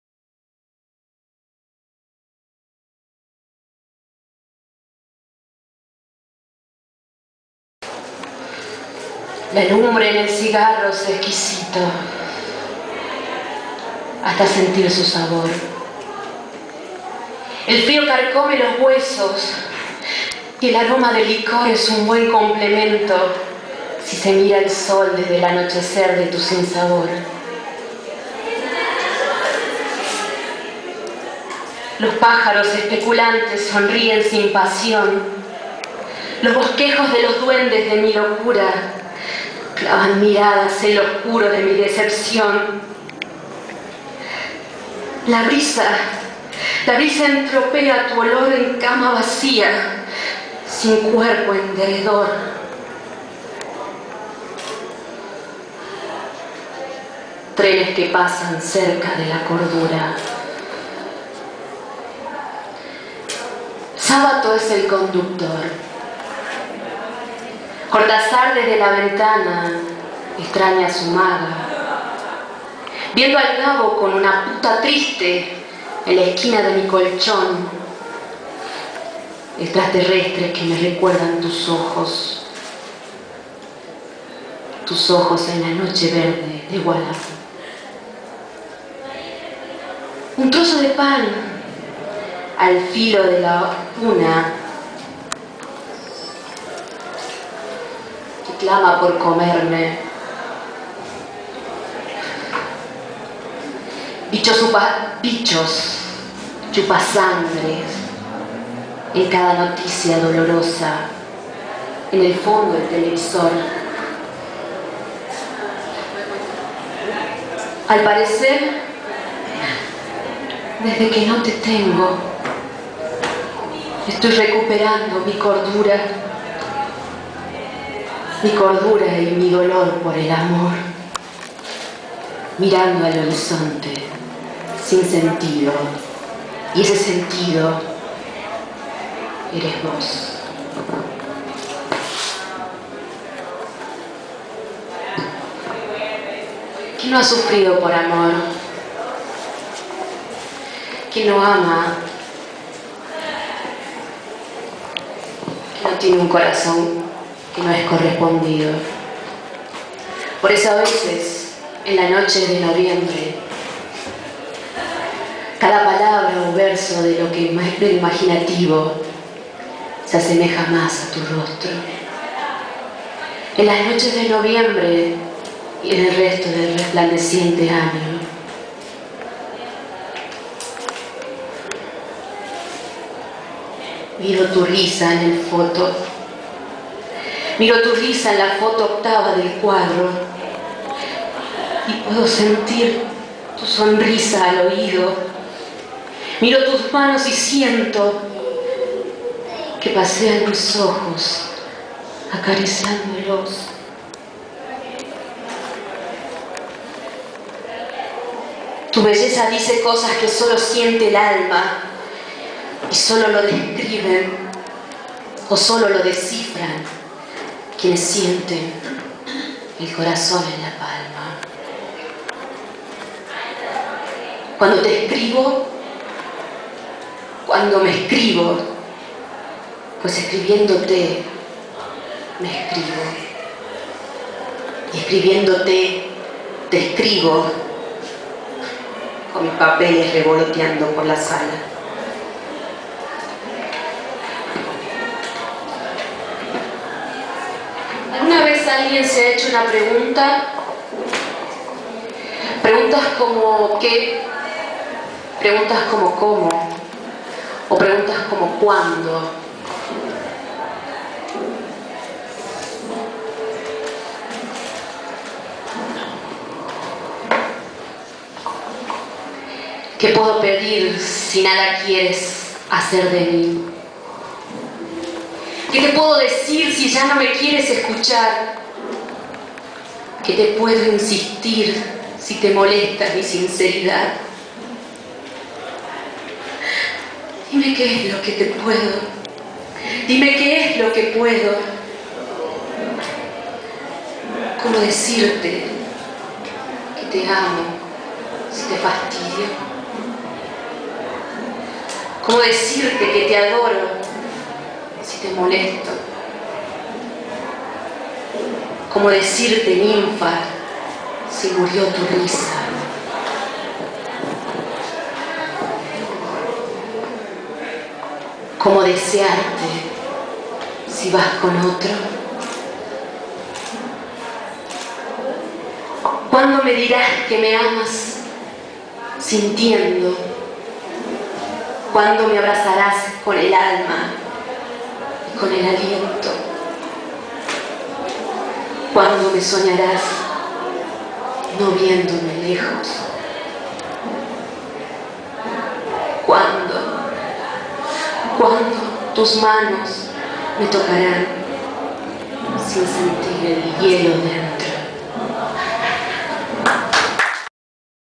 Anku ElectroCabaret en la Flia - Luz Sacchetti leyendo a Byron Torres
texto: Byron Torres

29 de marzo de 2013
La Sodería Teatro